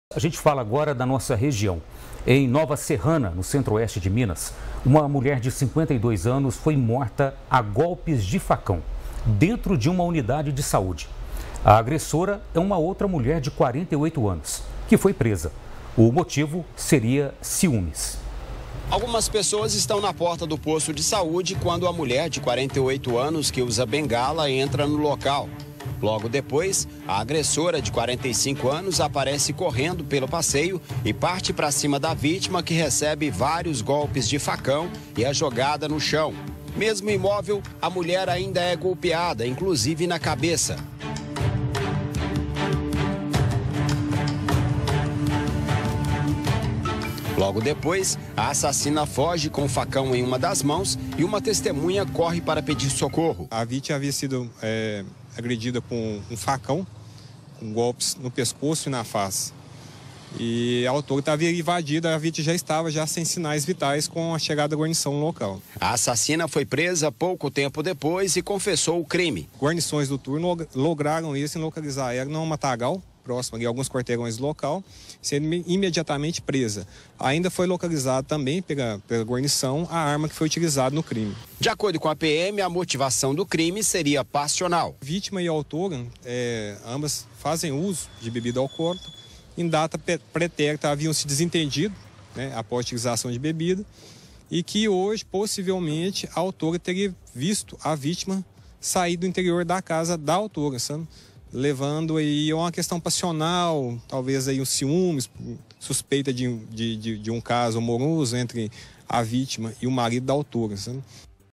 MULHER É ATACADA E MORTA A GOLPES DE FACÃO DENTRO DE UNIDADE DE SAÚDE DE NOVA SERRANA
| REGIÃO |

Em Nova Serrana, no centro oeste de Minas, uma mulher de 52 anos foi morta a golpes de facão, dentro de uma unidade de saúde. A agressora é uma outra mulher de 48 anos, que foi presa. O motivo seria ciúmes. Acompanhe todos os detalhes na reportagem: